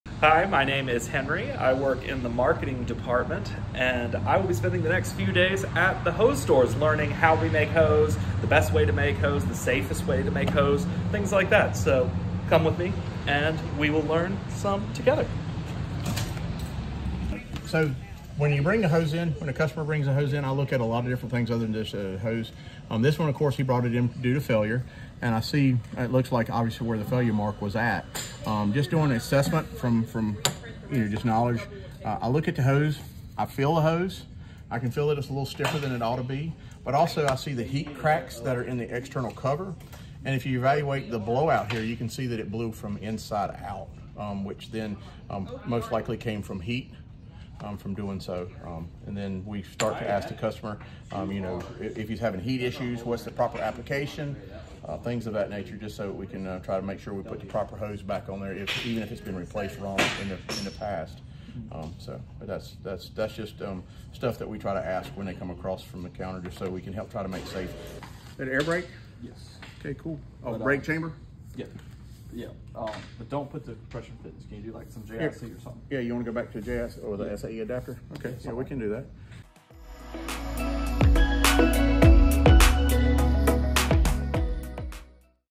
Hose Inspection | Hose Making with Henry
Henry’s journey continues! In today’s episode, we’re diving into pinpointing potential failures when assessing hoses. With a keen eye for detail and commitment to excellence, Henry is ready to provide customers with top-tier safety protocols and fabrication recommendations.
At Cross, we’re committed to providing our employees with the best training, so they can work safely and efficiently to provide the best service.